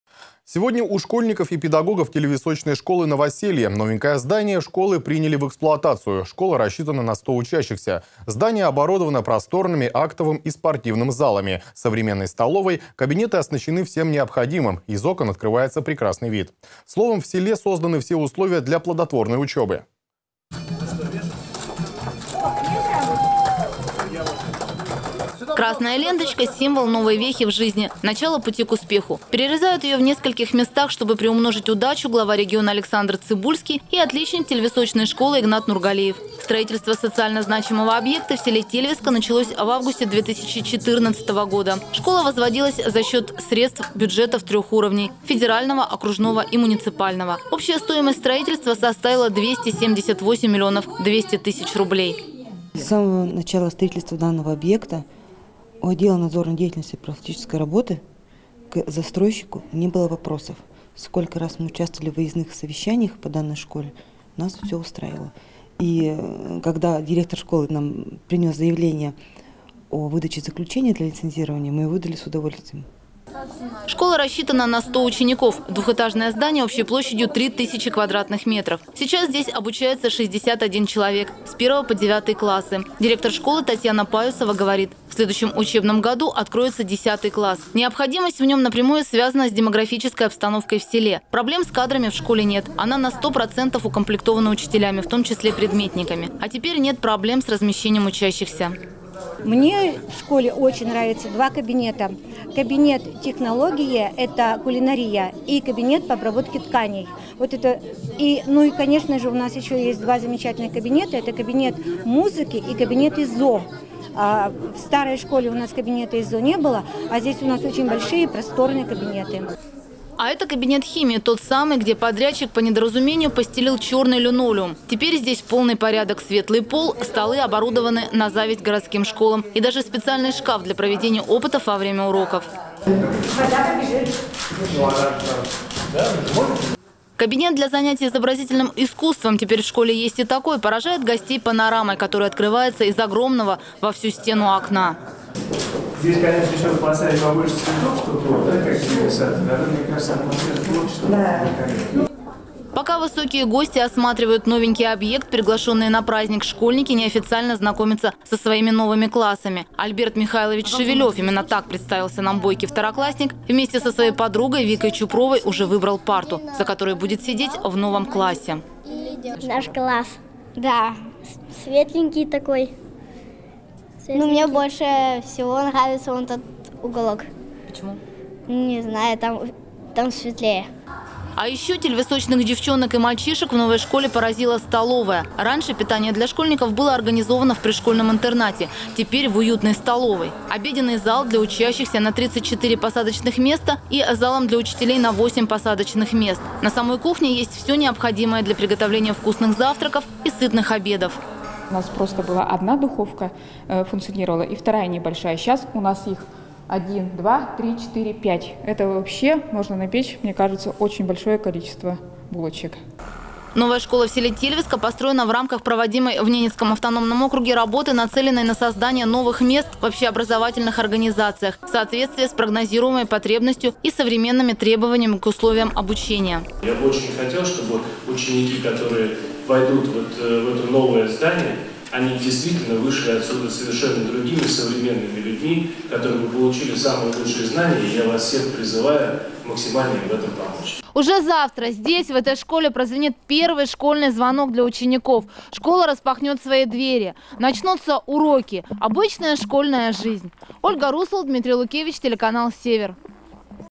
В Тельвиске открыла двери новая школа
Сегодня у школьников и педагогов Тельвисочной школы новоселье. Новенькое здание школы приняли в эксплуатацию. Школа рассчитана на 100 учащихся. Здание оборудовано просторными актовым и спортивным залами, современной столовой, кабинеты оснащены всем необходимым, из окон открывается прекрасный вид. Словом, в селе созданы все условия для плодотворной учебы.